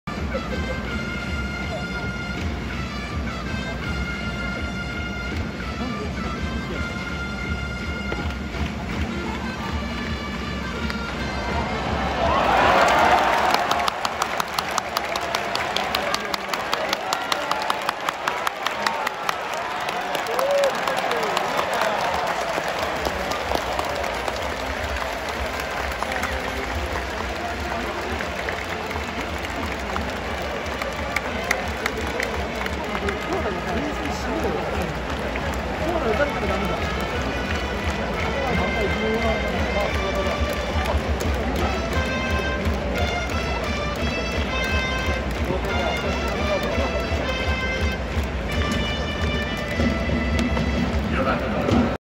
森友哉の同点タイムリーツーベースをフラッグの臨場感とともに【西武vs日本ハム】2022/8/31
2022年8月31日
埼玉西武ライオンズvs北海道日本ハムファイターズ
22回戦 ベルーナドーム

5回裏 ツーアウト1、2塁
3番 森友哉 試合を振り出しに戻し隅田知一郎の負けを消すタイムリー！
L 2-2 F